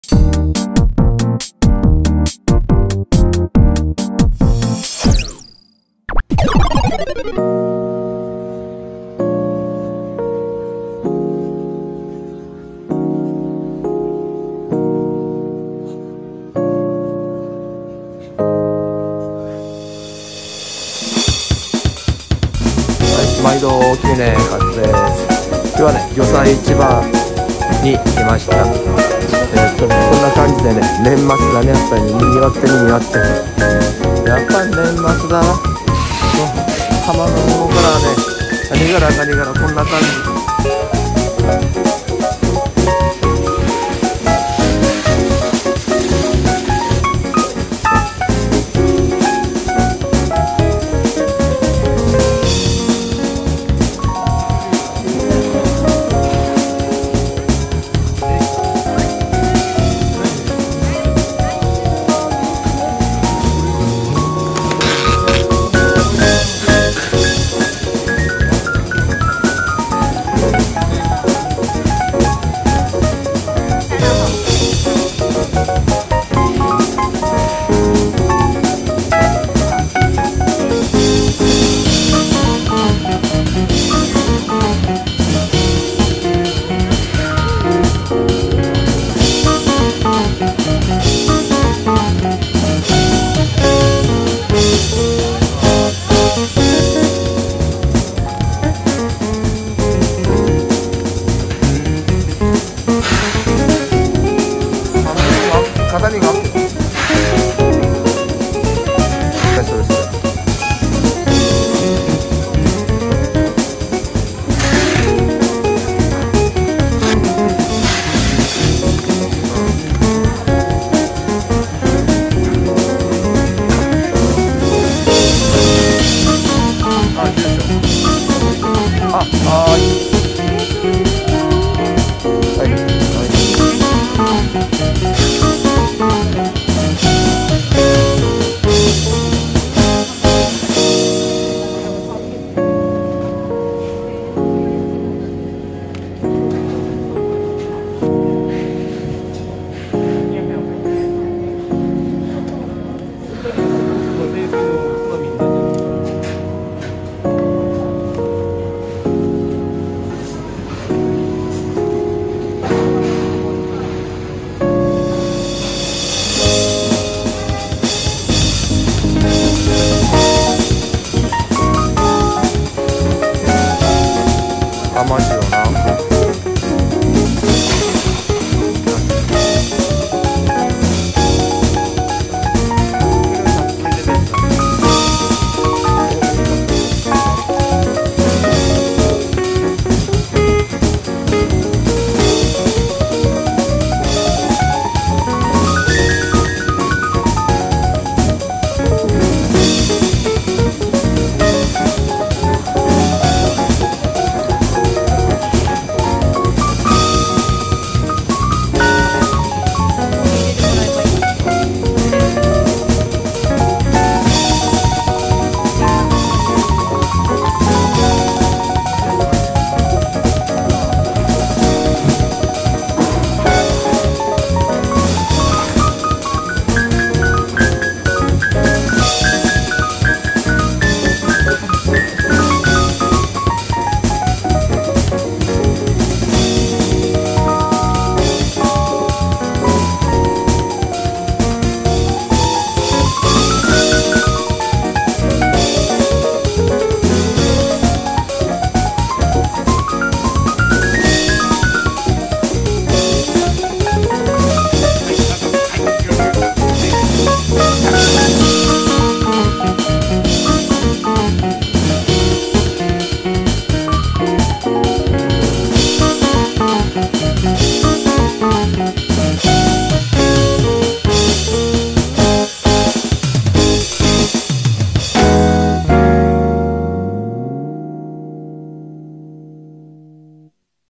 【年末動画】宮古魚菜市場（2022.12）
今回は贈り物しに宮古市魚菜市場にきました。
もうこういう動画は喋らない方がいいっすね。
撮影の３分ぐらい前には全く身動きとれないまま一周した後だったのですが、こんな感じでした。

BGM/MusMus